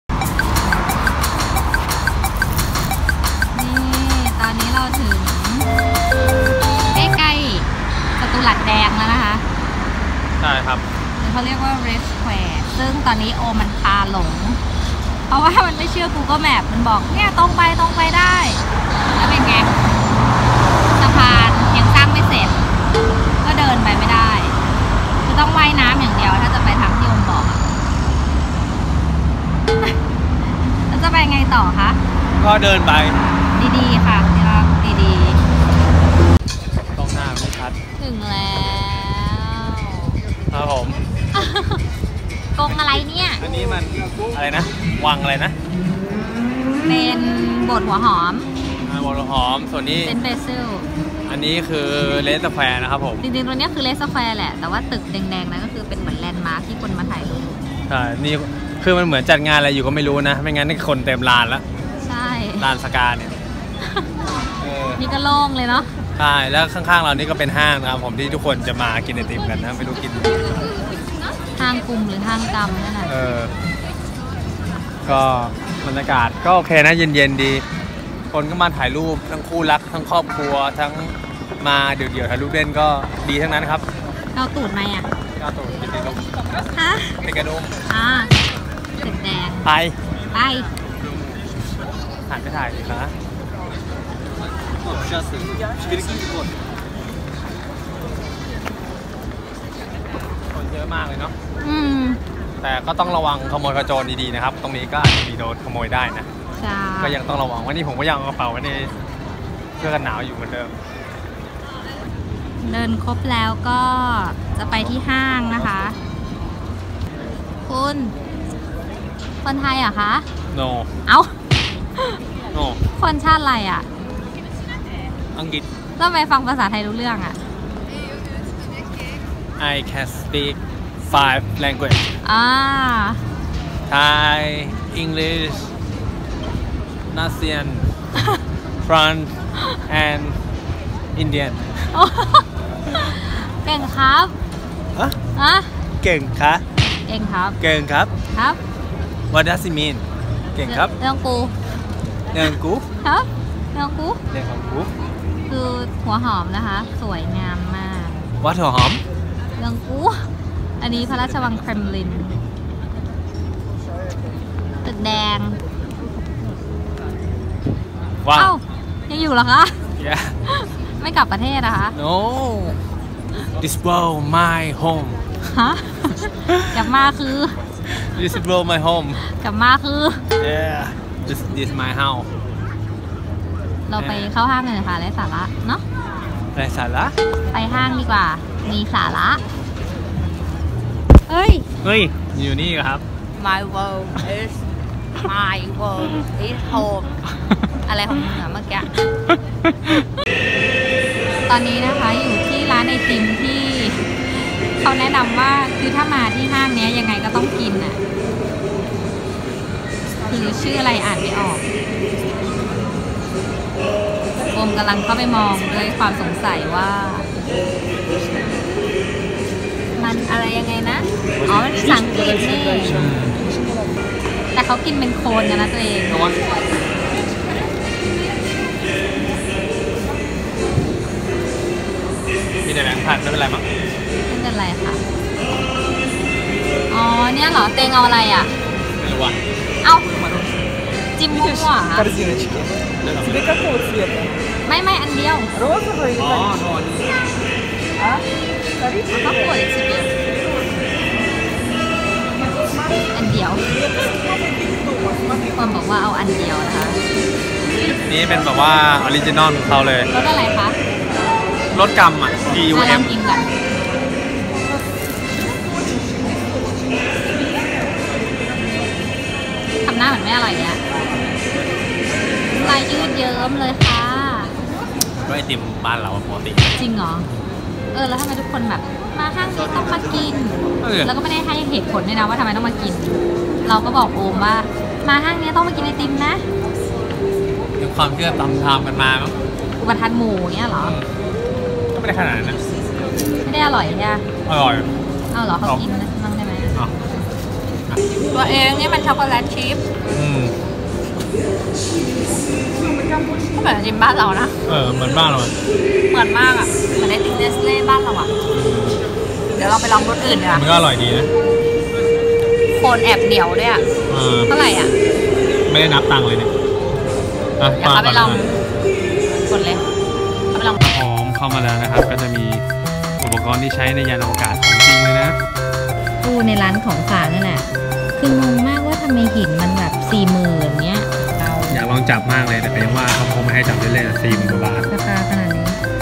ไอติมรัสเซีย สูตร100ปี รสชาติจะพิสดารขนาดไหน!? | เที่ยวรัสเซียด้วยตัวเอง EP4
ไอติมที่ทุกคนต้องมาลอง ใครไม่ได้กินถือว่าพลาดมากๆ!!!

ติดตามอัพเดตพวกเราได้ที่นี่เลยจ้า

😺คลิปแกล้งแมว เล่นกับแมว🐈

พูดคุย ลงโฆษณา เป็นกำลังใจกันได้ที่